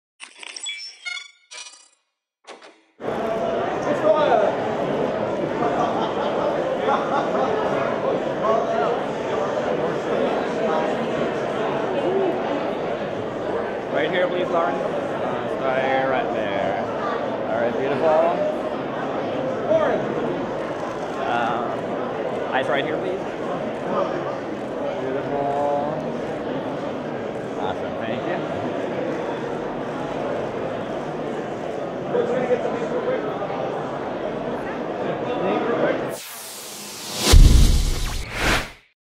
Lauren Phillips 2018 XBIZ Awards Red Carpet
Broll footage: Lauren Phillips walks the red carpet at the annual XBIZ Awards held at the JW Marriott at L.A. LIVE in Los Angeles, California USA on January 18, 2018